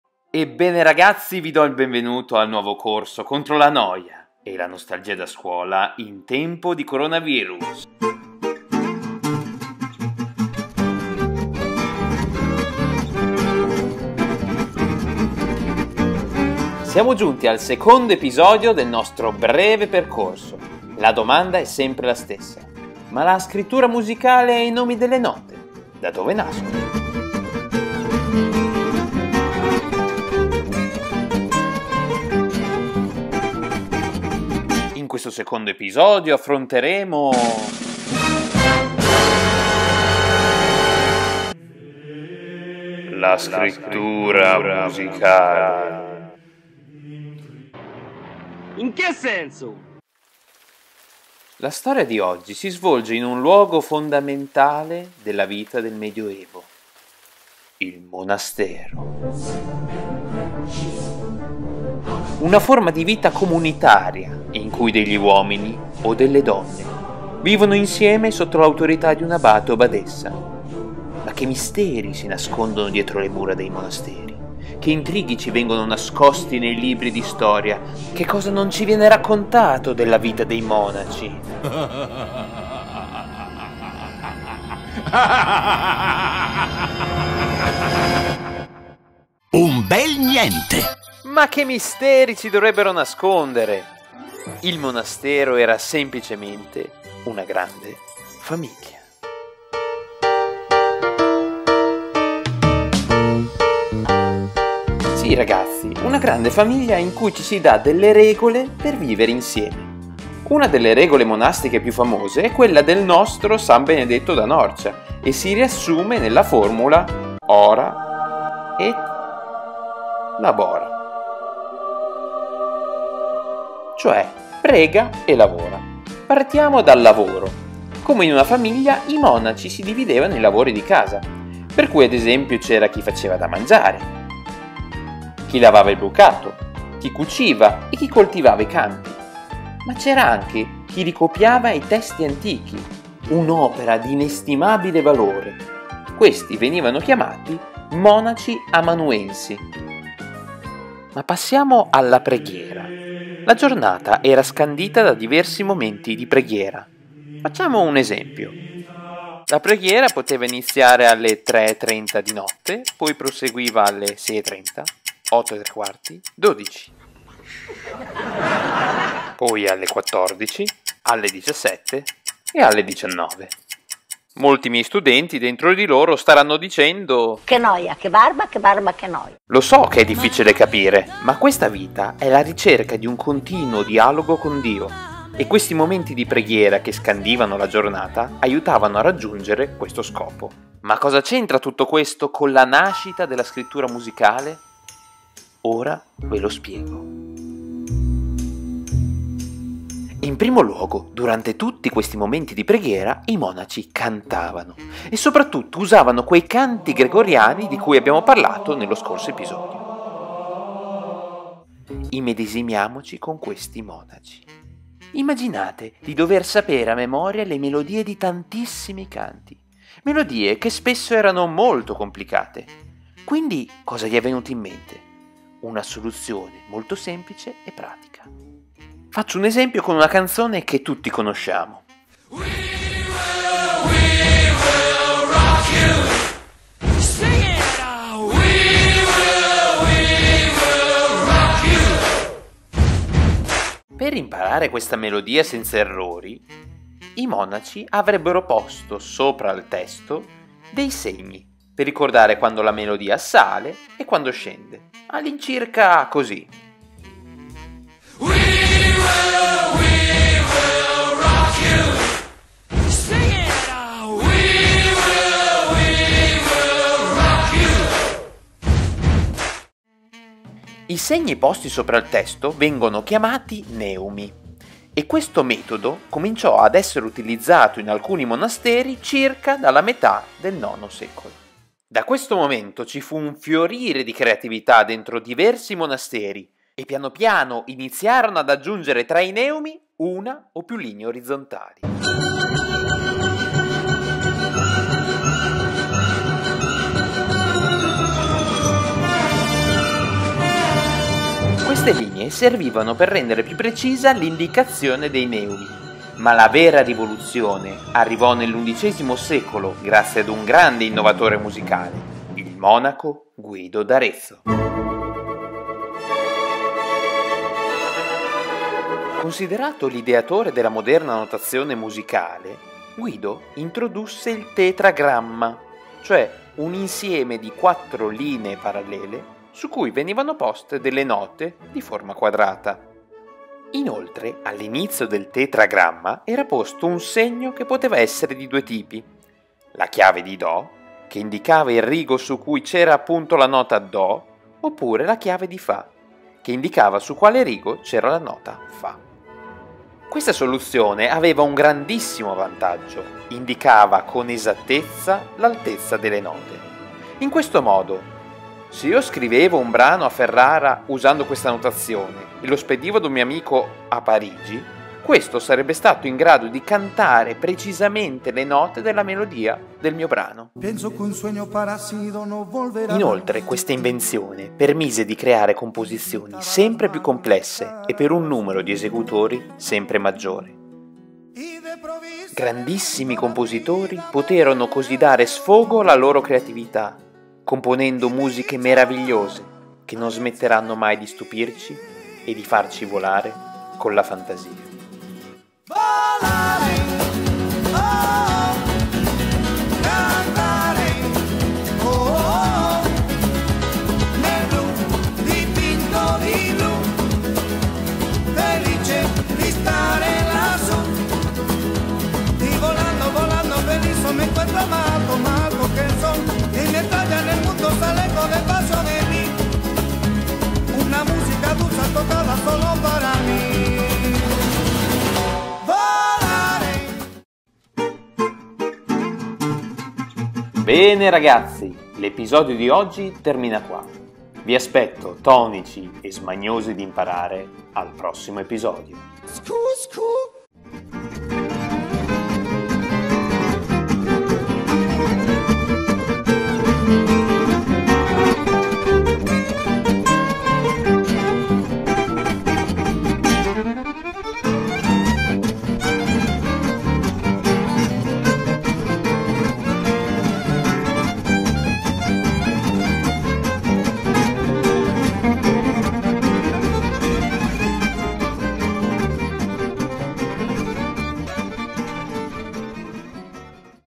ORIGINE DELLA SCRITTURA MUSICALE
Secondo episodio del nostro percorso in cui, in modo leggero e coinvolgente,  scopriamo la nascita della scrittura musicale. Adatto ad alunni e a chiunque sia curioso di scoprire le origini del nostro sistema di notazione.